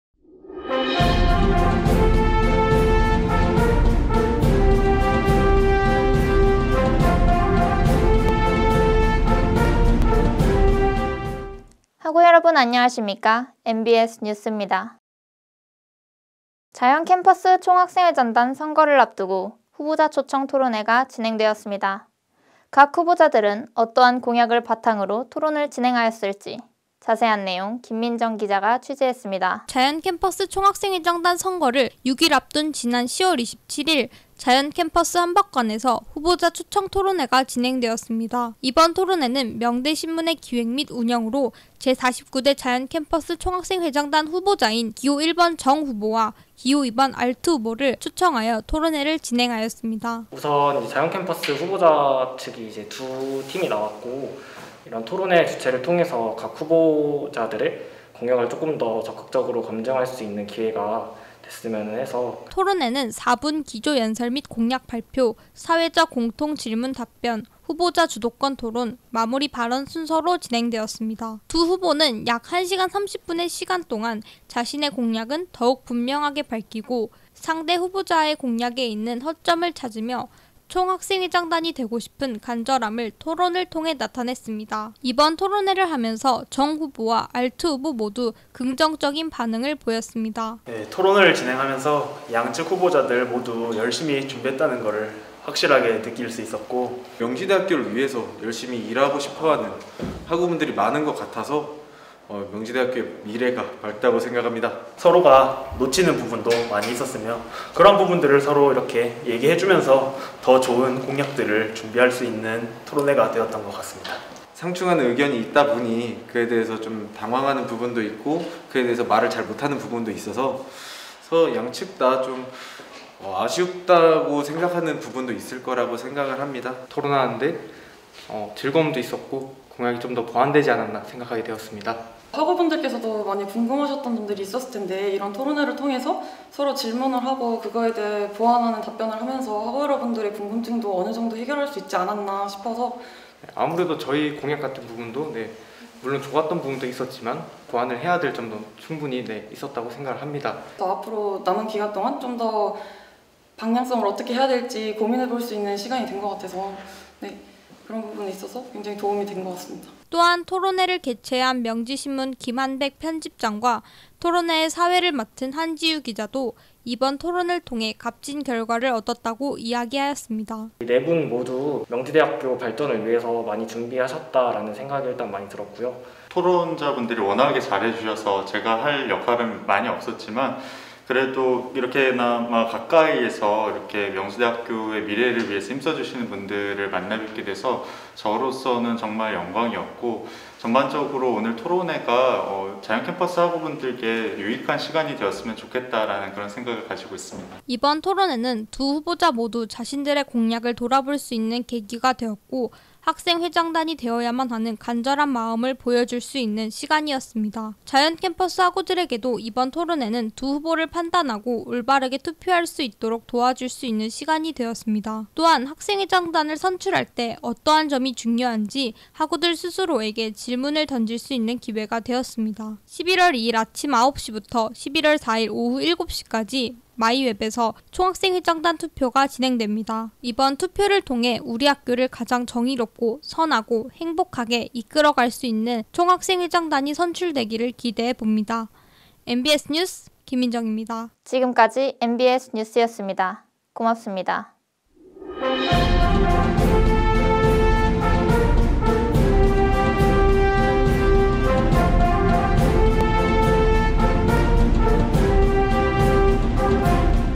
[MBS 뉴스] 21년 11월 03일
[MBS뉴스]

2021년 11월 03일

명대신문사 주관 제 49대 자연캠퍼스 총학생회장단 후보자 초청 토론회 개최
영상 촬영 및 취재 : MBS 보도부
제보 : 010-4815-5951
카카오톡 ID : mbs